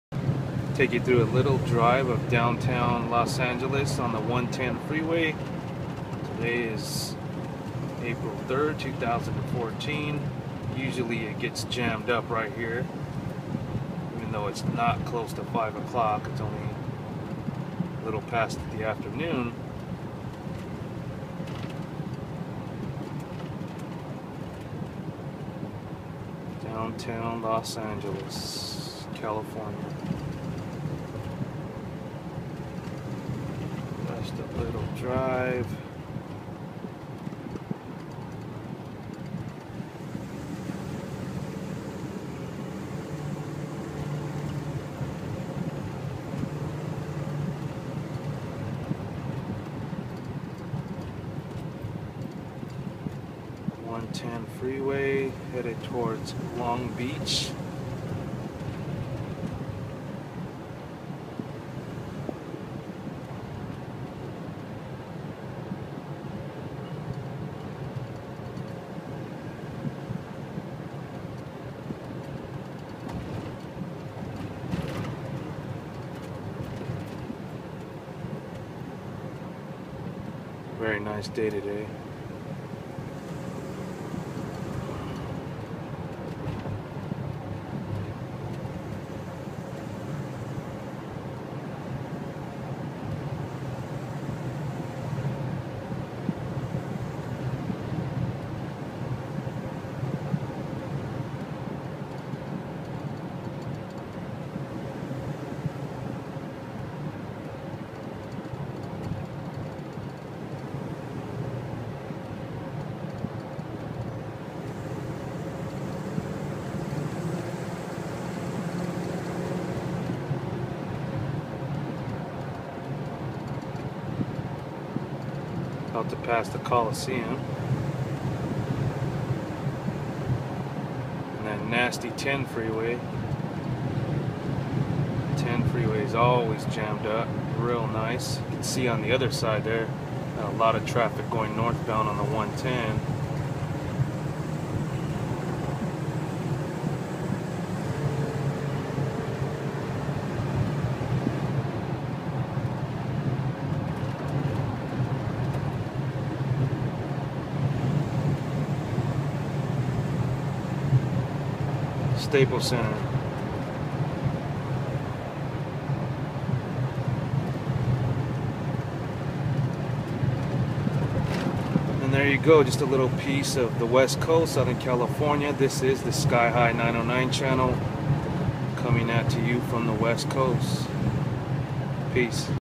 Downtown Los Angeles Drive
A drive on the 110 freeway to catch a glimpse of Downtown LA in April 2014.